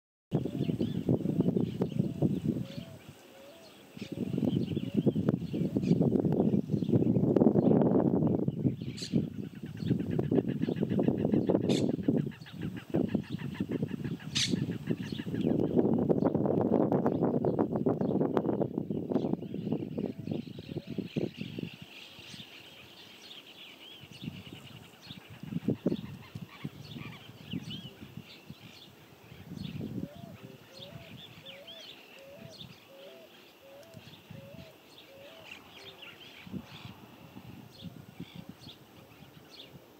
Lagarto Overo contra Patos Sirirí Pampa (9/10/22)
Dendrocygna viduata vs Salvator merianae (20221009)